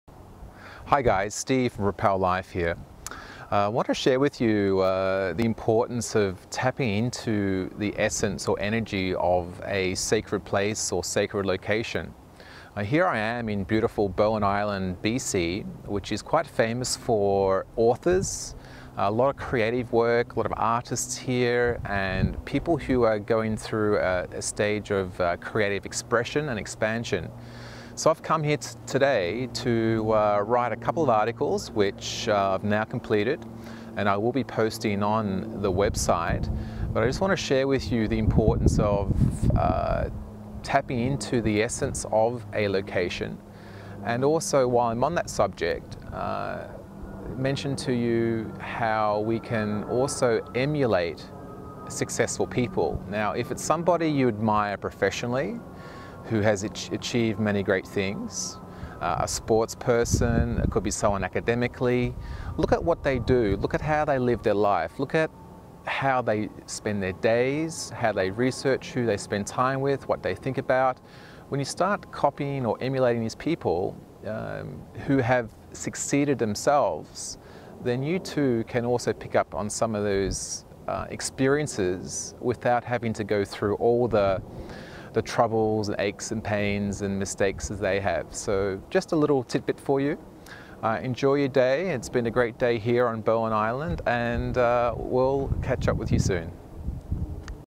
Create, Write and Copy Successful People from Propel Life on Bowen Island
Recoded on Bowen Island , B.C., Canada: Steve from Propel Life goes to Bowen Island to experience the Islands creativity and famous reputation for producing award winning authors.  Also, talks about the benefits of copying successful people to succeed yourself.